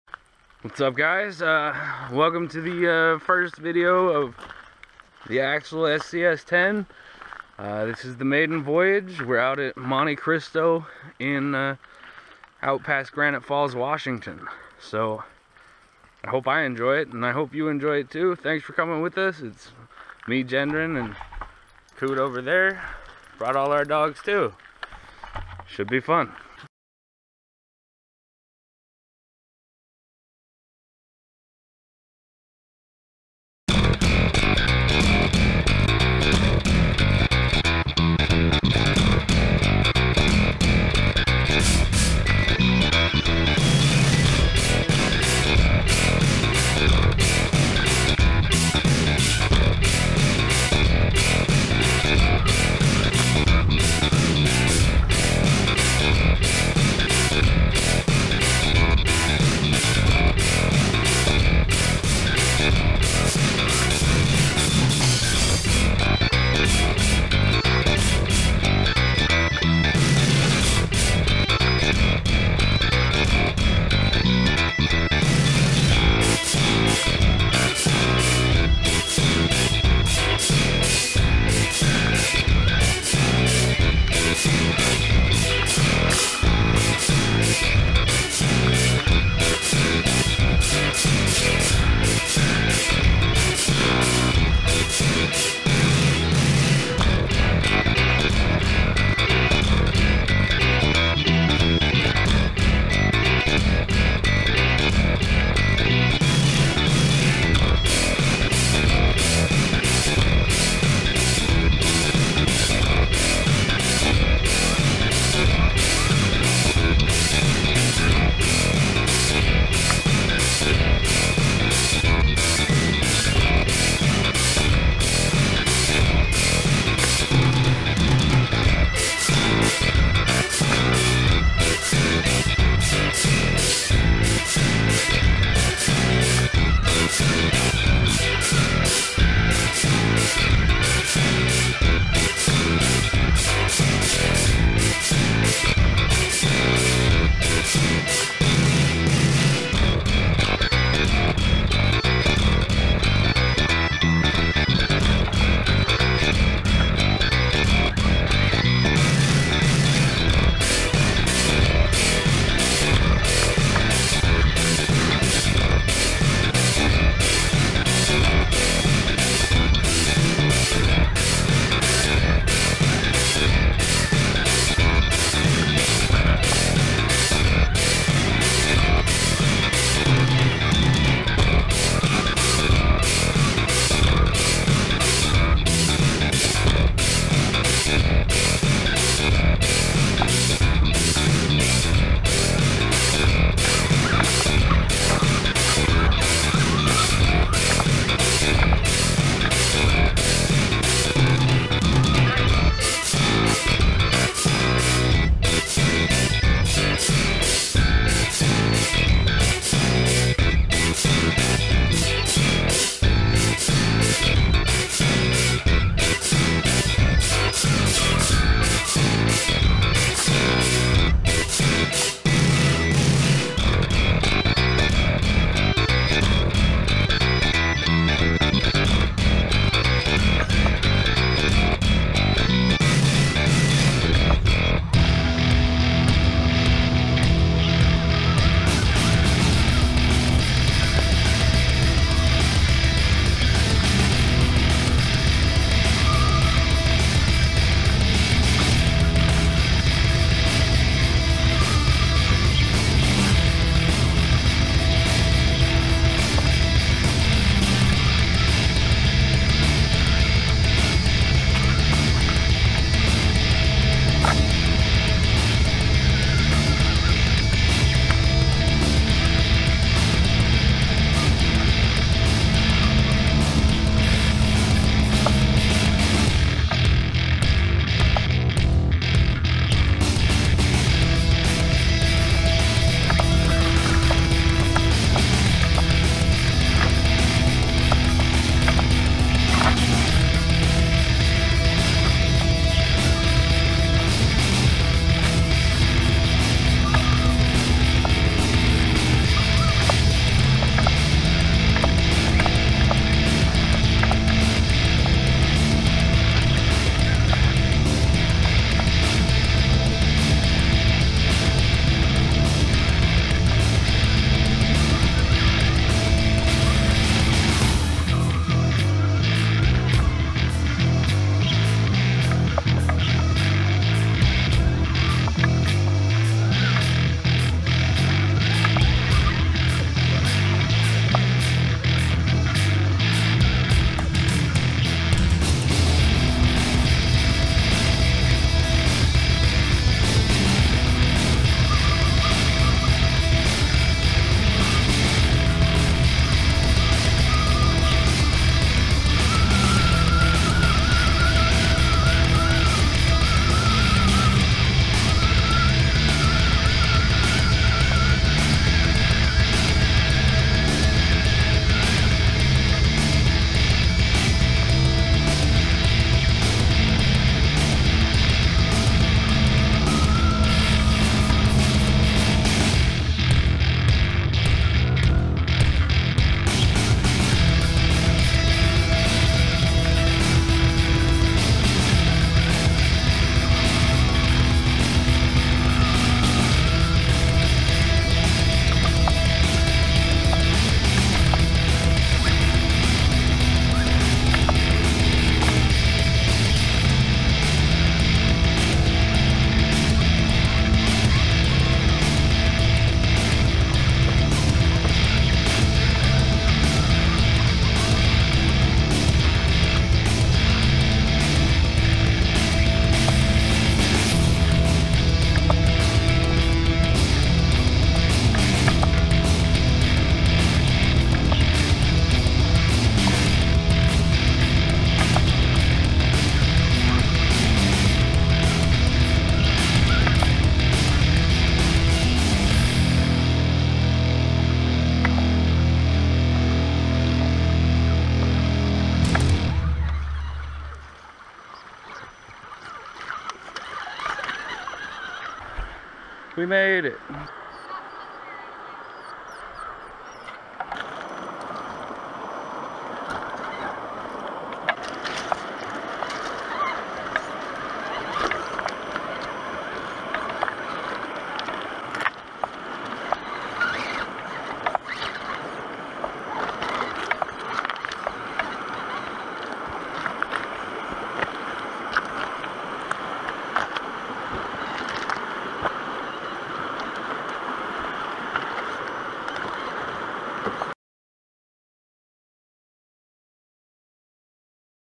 RC EXPEDITION - SCX10 Maiden voyage - Before Upgrading
Hey YouTubers It's me Gendron here went out for a hike in Monte Cristo WA. Brought the new SCX10 with to see how she did. well the performance was awesome way to go Axial. only issue was i lost a screw in my steering knuckle.